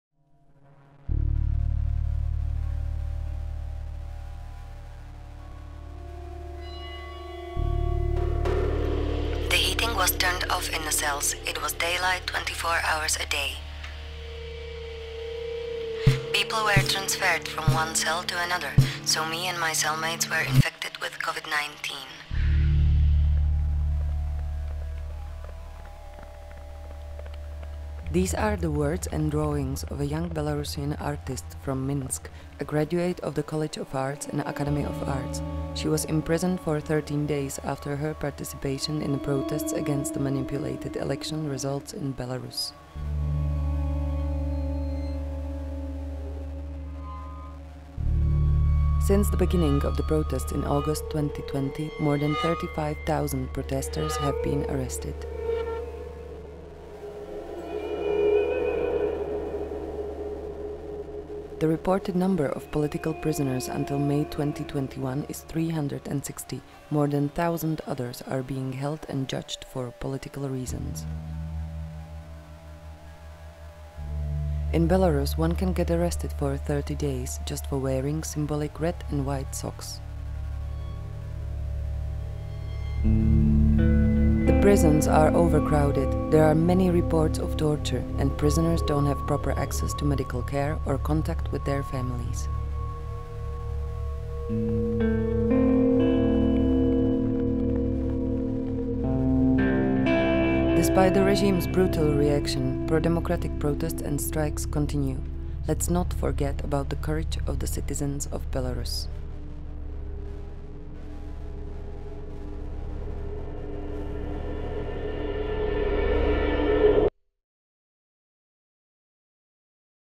FreeBelarus: Let's not forget about the courage of the citizens of Belarus
The author of the drawings that  we used in the video wrote to us:

"I'm an artist. I was born in Minsk. I graduated from College of Arts and Academy of Arts. I mostly paint using oil, sometimes I do graphics. When the elections were held, I was so shocked by the events that followed, that for several months I wasn't able to do art at all. I had drawn almost nothing until the moment when I went through detention and an administrative arrest. Once I had experienced this, I felt that now I'm able to tell about it through my artwork. And I thought it was important to do this because this story is one of many dozens of thousands of people stories who had a similar experience. It all started with the sketches that I made in the prison cells that I was placed to during my 13-day arrest. These sketches to a greater extent show the life of a political prisoner, while the drawings that I made after I was released, reproduce my emotional experience. There are many stories related to the arrest under the same article, which are much more terrible and tragic. My arrest was relatively calm and yet made a rather strong impression to me. During my arrest, the heating was turned off in the cells, daylight was on twenty-four hours a day, measures to prevent the spread of Covid-19 were not only not followed, but on the contrary, people were transfered from one cell to another cell, so me and my cellmates were infected, but we know that many people who were under arrest prior to me, and those who are imprisoned now, they are facing much more terrible conditions..."